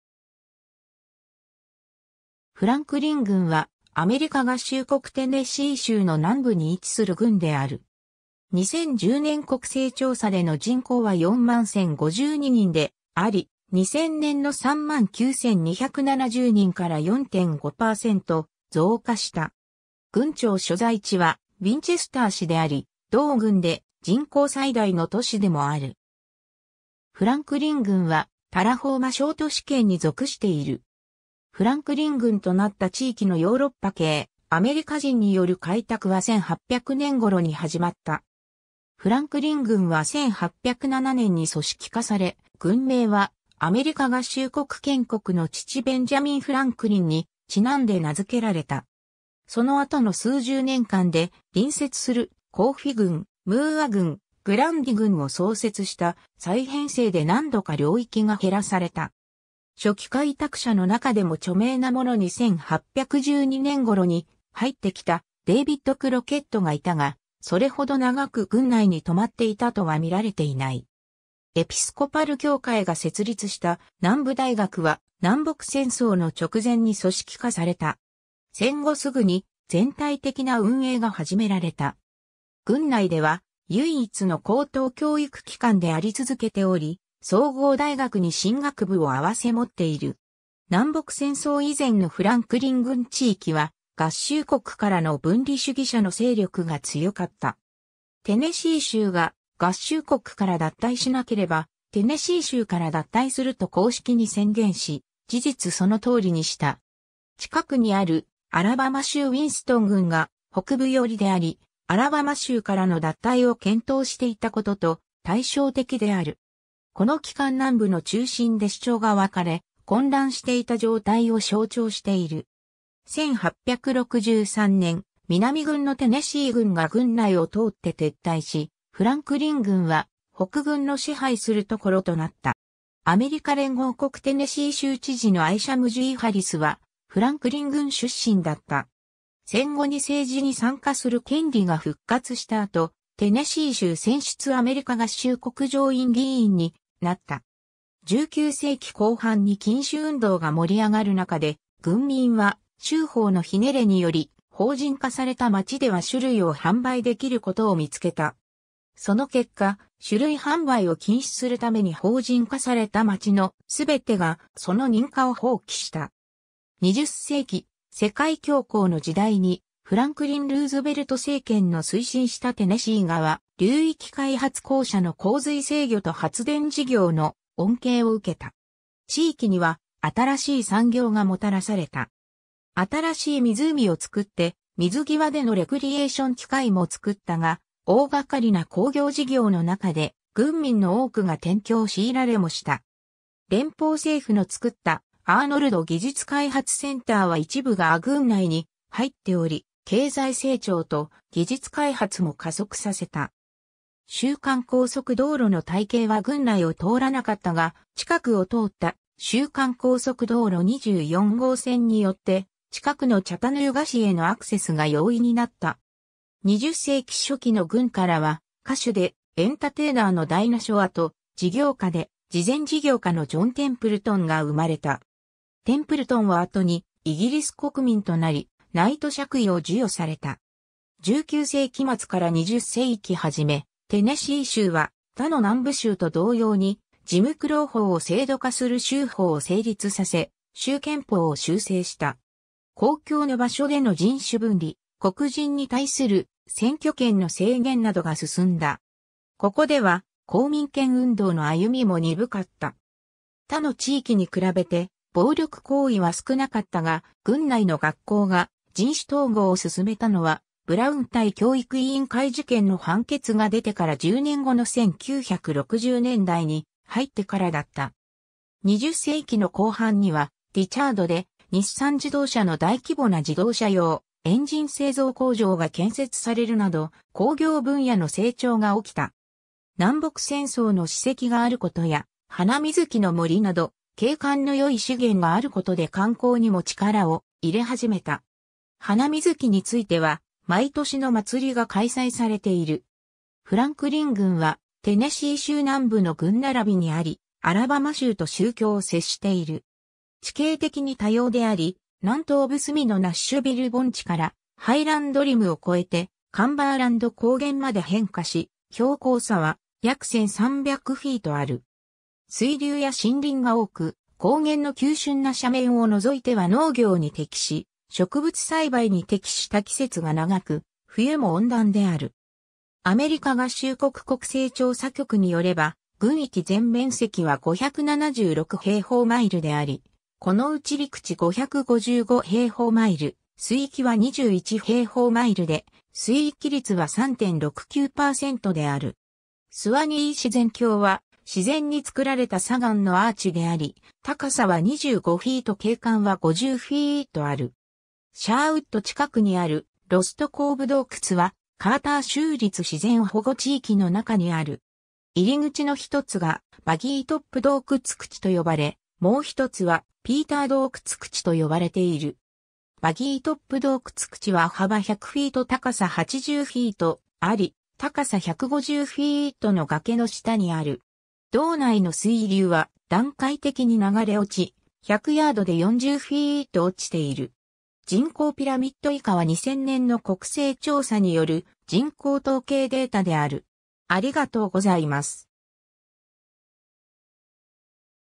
フランクリン郡 (テネシー州)
ベンジャミン・フランクリン
フランクリン郡（英: Franklin County）は、アメリカ合衆国テネシー州の南部に位置する郡である。
2010年国勢調査での人口は41,052人であり、2000年の39,270人から4.5%増加した。
郡庁所在地はウィンチェスター市（人口8,530人）であり、同郡で人口最大の都市でもある。
 フランクリン郡はタラホーマ小都市圏に属している。
 フランクリン郡となった地域のヨーロッパ系アメリカ人による開拓は1800年頃に始まった。
フランクリン郡は1807年に組織化され、郡名はアメリカ合衆国建国の父ベンジャミン・フランクリンに因んで名付けられた。
その後の数十年間で、隣接するコーフィ郡、ムーア郡、グランディ郡を創設した再編成で何度か領域が減らされた。
初期開拓者の中でも著名な者に1812年頃に入ってきたデイヴィッド・クロケットがいたが、それほど長く郡内に留まっていたとは見られていない。
 エピスコパル教会が設立した南部大学は南北戦争の直前に組織化された。
戦後直ぐに全体的な運営が始められた。
郡内では唯一の高等教育機関であり続けており、総合大学に神学部を併せ持っている。
 南北戦争以前のフランクリン郡地域は合衆国からの分離主義者の勢力が強かった。
テネシー州が合衆国から脱退しなければテネシー州から脱退すると公式に宣言し、事実その通りにした。
近くにあるアラバマ州ウィンストン郡が北部寄りであり、アラバマ州からの脱退を検討していたことと対照的である。
この期間南部の中心で主張が分かれ、混乱していた状態を象徴している。
 1863年、南軍のテネシー郡が郡内を通って撤退し、フランクリン郡は北軍の支配するところとなった。
アメリカ連合国テネシー州知事のアイシャム・G・ハリスはフランクリン郡出身だった。
戦後に政治に参加する権利が復活した後、テネシー州選出アメリカ合衆国上院議員になった。
 19世紀後半に禁酒運動が盛り上がる中で、郡民は州法の捻れにより、法人化された町では酒類を販売できることを見つけた。
その結果、酒類販売を禁止するために法人化された町の全てがその認可を放棄した。
 20世紀、世界恐慌の時代にフランクリン・ルーズベルト政権の推進したテネシー川流域開発公社の洪水制御と発電事業の恩恵を受けた。
地域には新しい産業がもたらされた。
新しい湖を造って水際でのレクリエーション機会も造ったが、大がかりな工業事業の中で、郡民の多くが転居を強いられもした。
連邦政府の作ったアーノルド技術開発センターは一部が郡内に入っており、経済成長と技術開発も加速させた。
州間高速道路の体系は郡内を通らなかったが、近くを通った州間高速道路24号線によって近くのチャタヌーガ市へのアクセスが容易になった。
 20世紀初期の郡からは歌手でエンタテイナーのダイナ・ショアと、事業家で慈善事業家のジョン・テンプルトンが生まれた。
テンプルトンは後にイギリス国民となり、ナイト爵位を授与された。
 19世紀末から20世紀初め、テネシー州は他の南部州と同様にジム・クロウ法を制度化する州法を成立させ、州憲法を修正した。
公共の場所での人種分離、黒人に対する選挙権の制限などが進んだ。
ここでは公民権運動の歩みも鈍かった。
他の地域に比べて暴力行為は少なかったが、郡内の学校が人種統合を進めたのは「ブラウン対教育委員会事件」の判決が出てから10年後の1960年代に入ってからだった。
 20世紀の後半には、ディチャードで日産自動車の大規模な自動車用エンジン製造工場が建設されるなど、工業分野の成長が起きた。
南北戦争の史跡があることや、ハナミズキの森など景観のよい資源があることで観光にも力を入れ始めた。
ハナミズキについては毎年の祭が開催されている。
 フランクリン郡はテネシー州南部の郡並びにあり、アラバマ州と州境を接している。
地形的に多様であり、南東部隅のナッシュビル盆地からハイランドリムを越えて、カンバーランド高原まで変化し、標高差は約1,300フィート (400 m) ある。
水流や森林が多く、高原の急峻な斜面を除いては農業に適し、植物栽培に適した季節が長く、冬も温暖である。
 スワニー自...